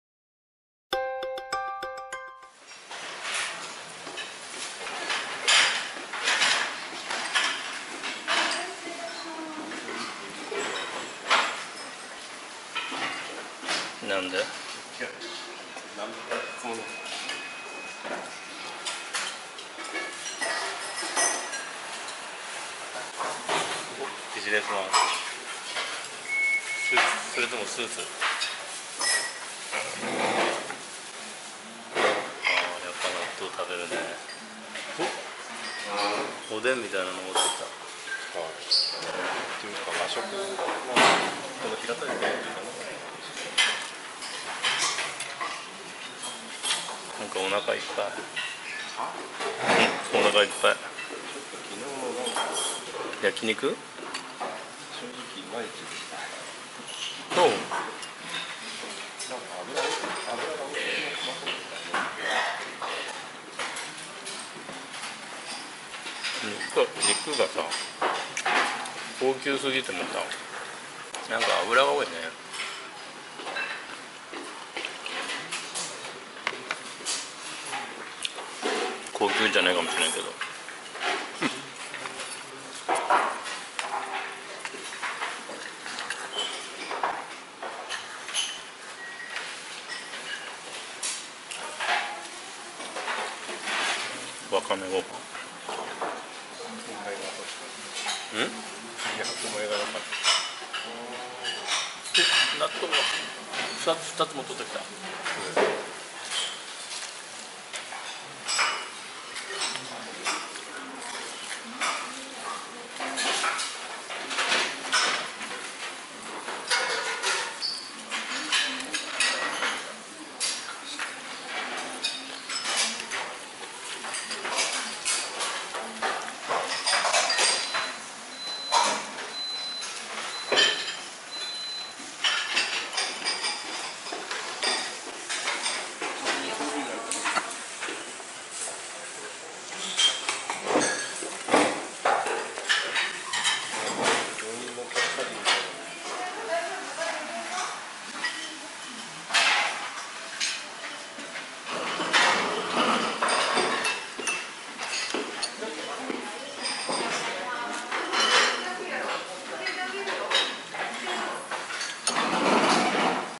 ホテルルートイン佐賀駅前朝食バイキング 佐賀県の旅
佐賀県の旅で、佐賀市のルートインに泊まりました。恒例の朝食バイキングです。